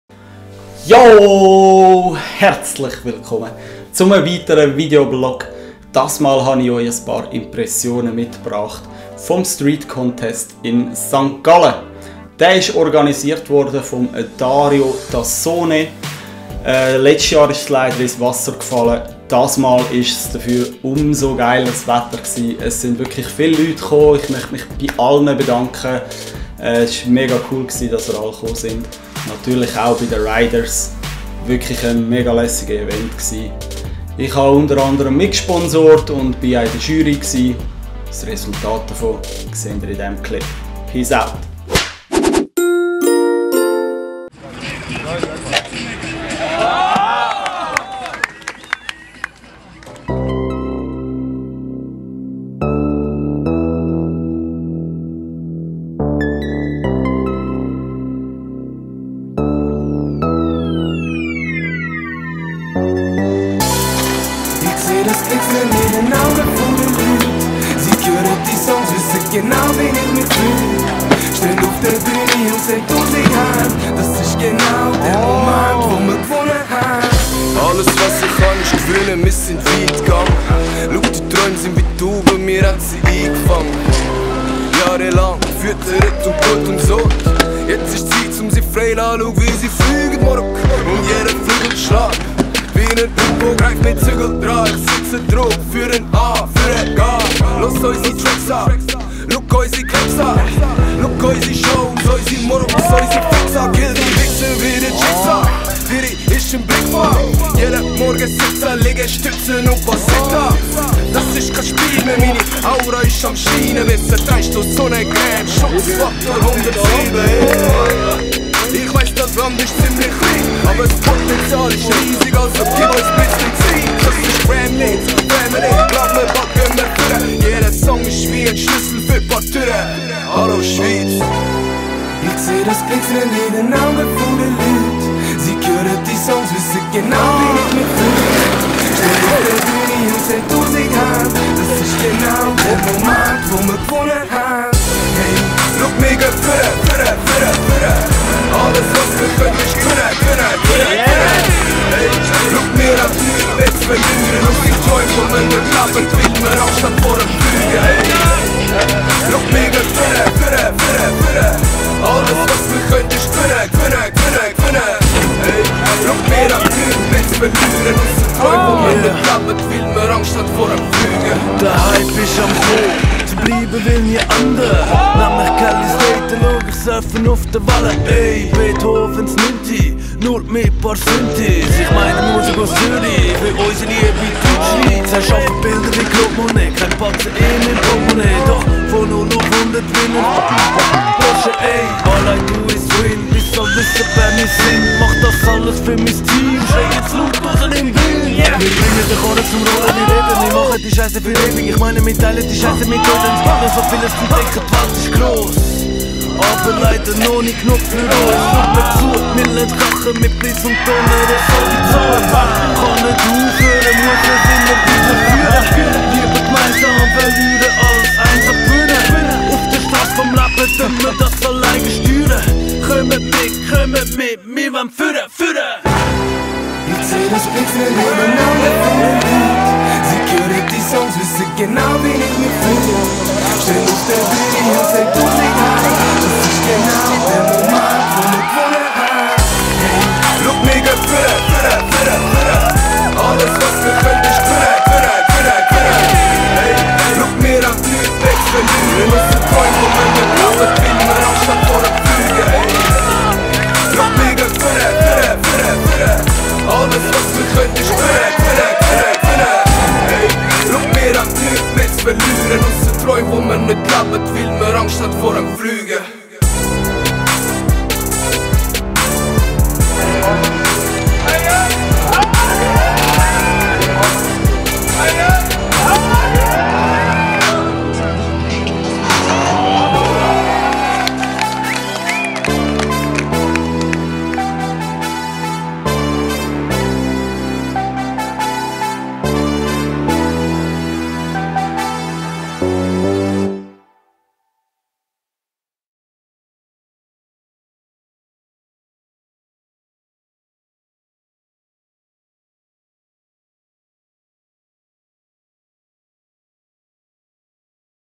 ✌️ Aggressive Inline Street Contest St. Gallen (snipClip 100 | Vlog🇨🇭)
Am Samstag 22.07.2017 fand der Rollerblading Streetcontest ST.Gallen statt. Hier gibt’s paar Eindrücke des Events/Wettkampfs.

Herzlichen Dank an alle die gekommen sind und natürlich einen GROSSEN Dank an Dario Tassone der das ganze Organisiert hat.

Fahrer die zu sehen sind in diesem Video:
Adrian Deck, Adriano Landsberg, Beat Schillmeier, Benjamin Wehrli, Clark Zeller, Diego Luppi, Jan Fehlmann, Karmi Schäfer, Kris Markiewicz, Lukas Burgunder, Mael Huguelet, Michael Müller, Nathan Akongo, Reto Bürgin, Siljan Sussmann, Stéphane Ryter

Jury Mitglieder:
Daniel Huber, Dario Tassone, Dean Bradley und Martin Bommeli

▬ Contest Resultate ▬▬▬▬▬▬▬▬▬▬▬
Pro:
1 Beat Schillmeier
2 Jan Fehlmann
3 Karmi Schäfer

AM:
1 Adriano Landsberg
2 Mael Huguelet
3 Benj Wehrli

▬ Verwendete Inhalte ▬▬▬▬▬▬▬▬▬▬▬▬
- Clips von Mir (snipClip 100, GoPro und Dji Mavic Drohne)